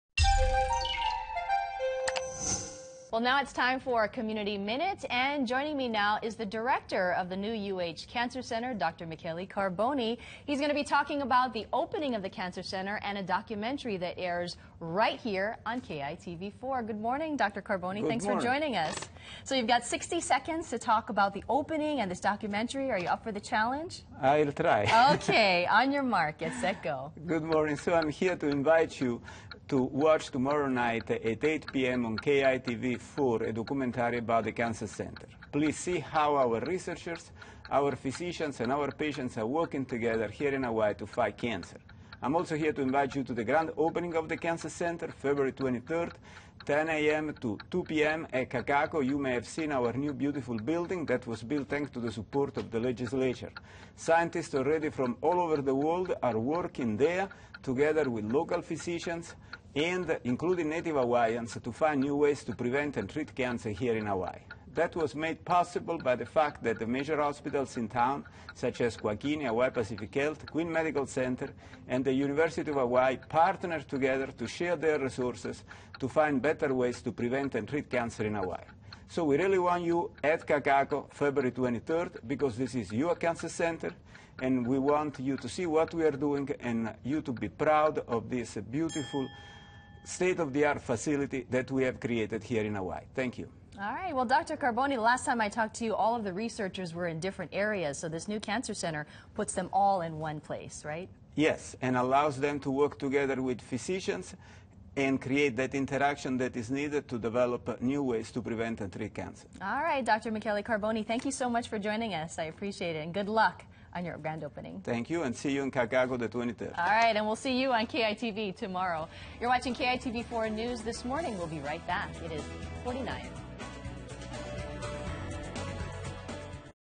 UH Cancer Center airs documentary
The University of Hawaii Cancer Center will be airing a 30-minute documentary on its new cancer center in Kakaako. It airs tomorrow at 8 pm on KITV4. For news you need to know,